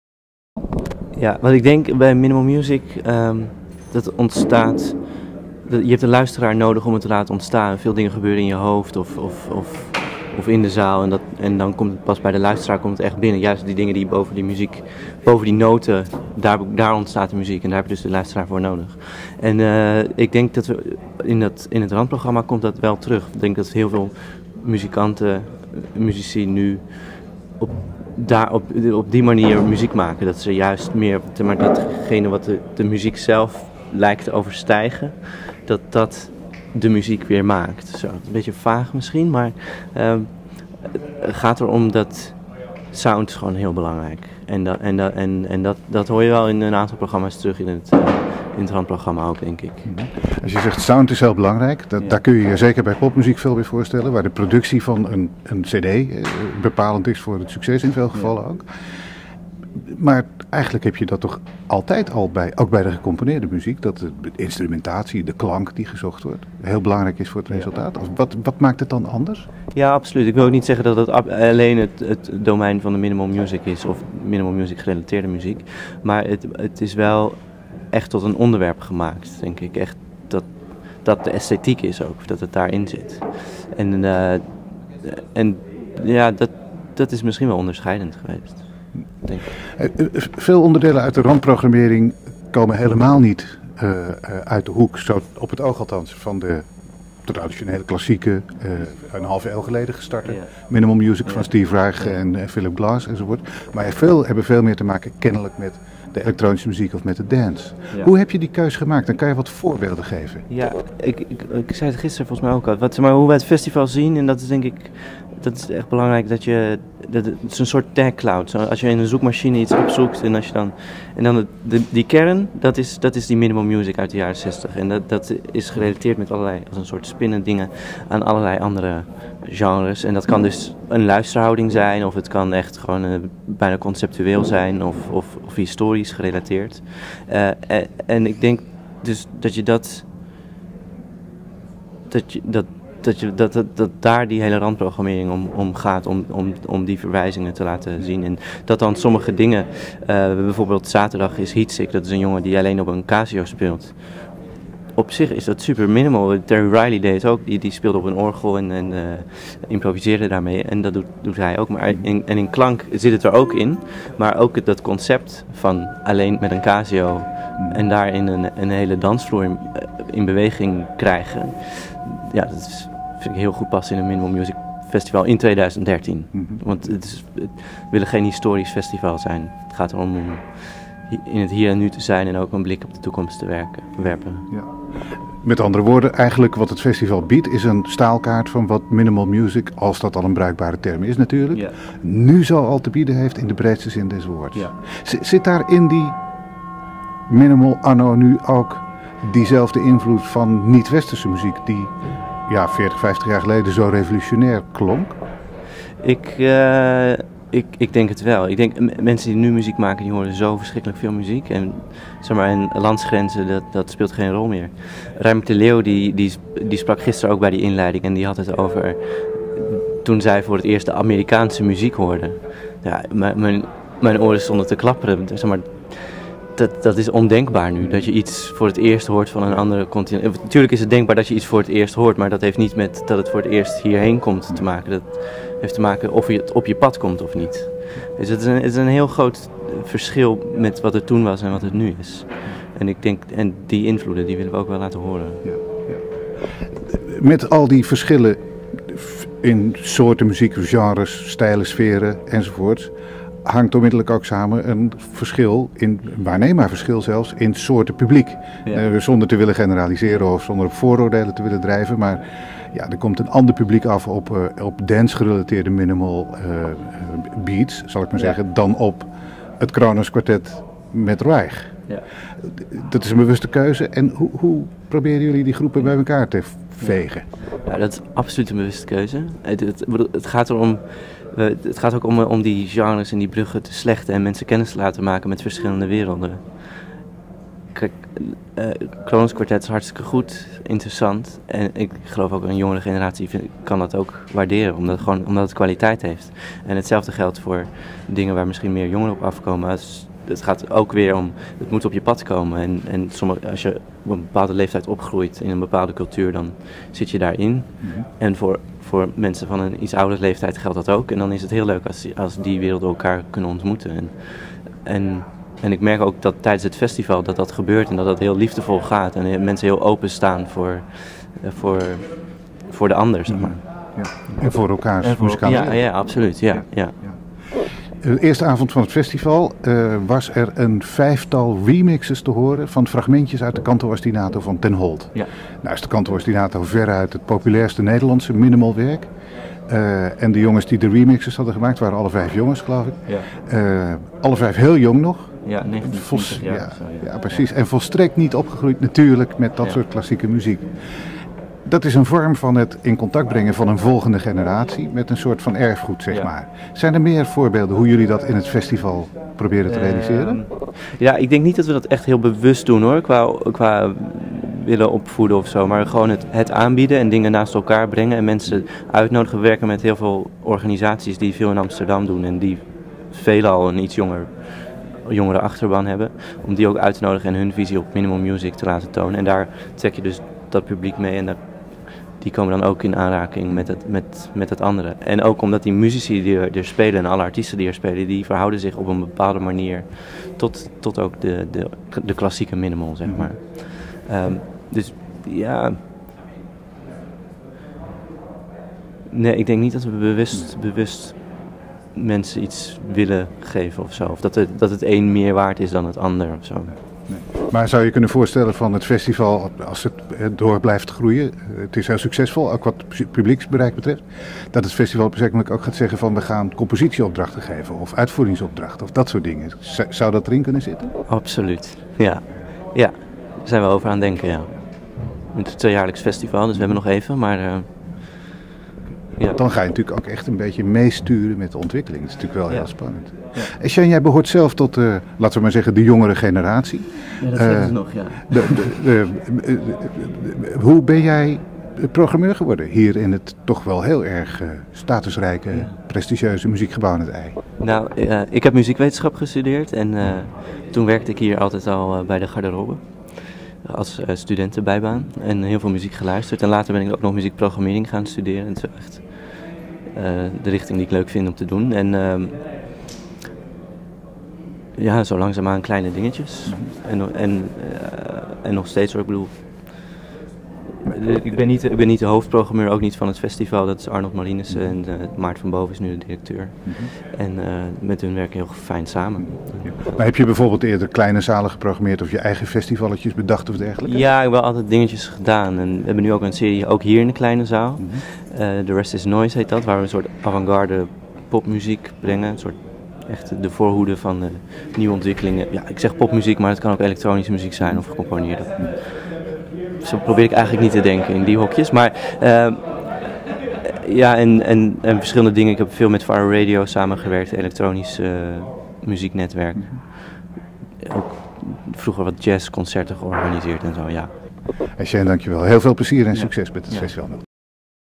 Shane Burmania
Shane Burmania, programmeur van de randprogrammering van het World Minimal Music Festival 2013.